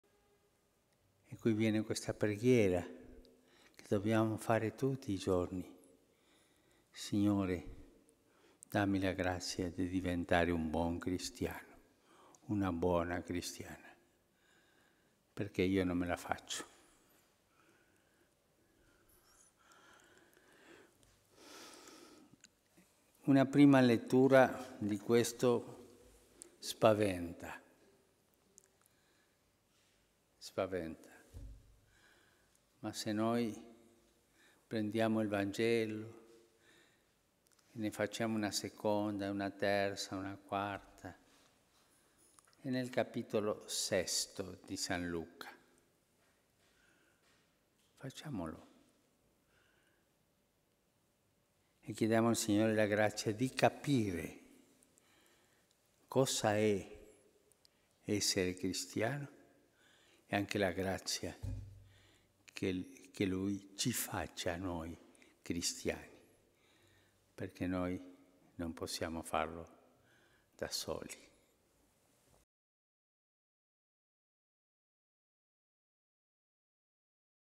Messa Santa Marta 11-09-2014
Amare i nemici, ce lo chiede Gesù

Papa Francesco, nell’omelia della messa del mattino celebrata a Casa Santa Marta, ha sottolineato che solo con un cuore misericordioso potremo davvero seguire Gesù, perché  la vita cristiana “non è autoreferenziale”, ma è dono fino alla fine, senza egoismo. Solo così sarà possibile amare senza confini, amare anche i nemici.